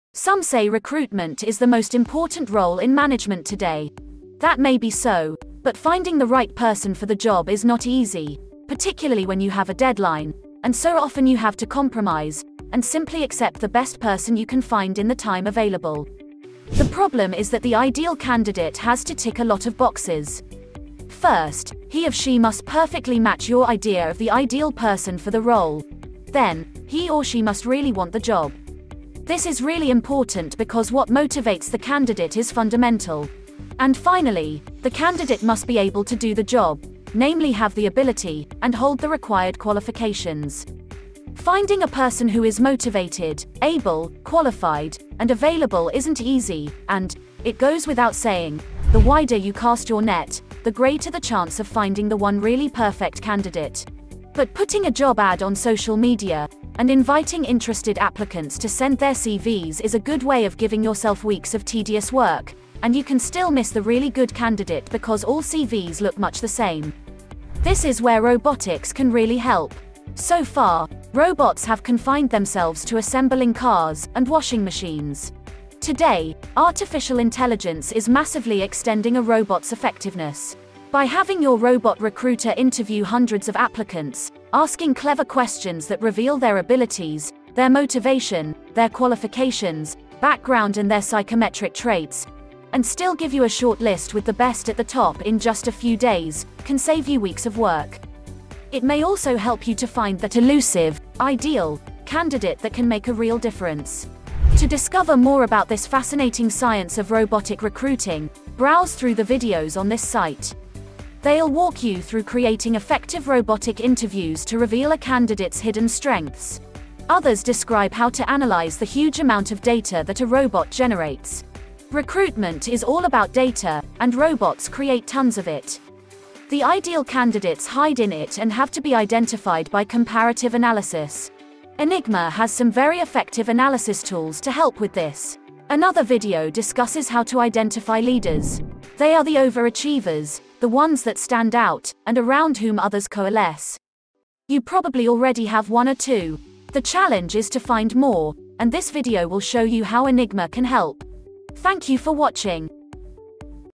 Recruitment Using AI
This video explains how AI -Powered recruitment makes recruitment easier and efficient.